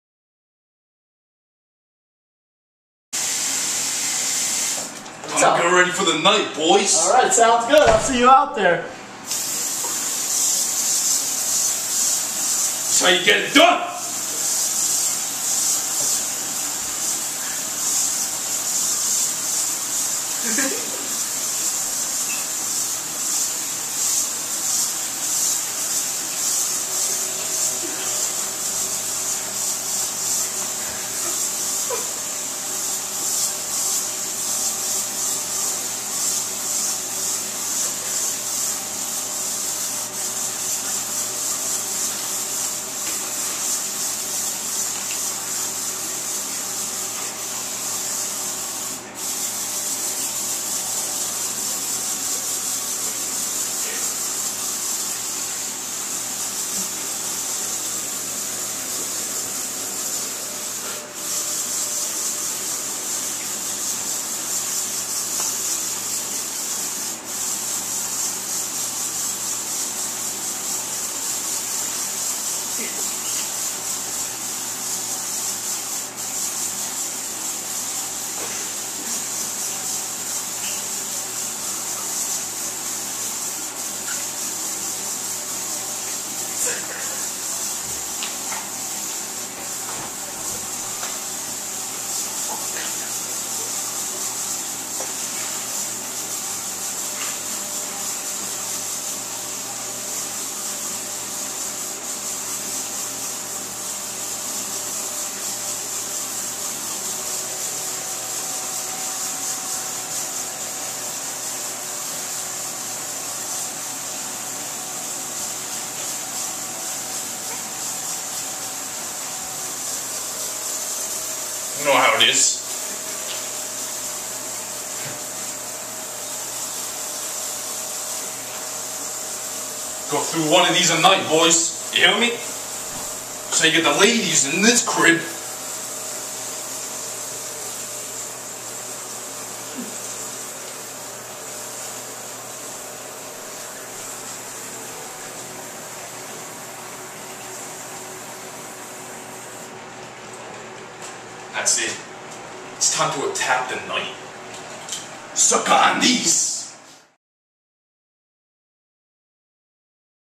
The Oracle Axe Blowout
the oracle prepares for a long night out,  gotta smell fresh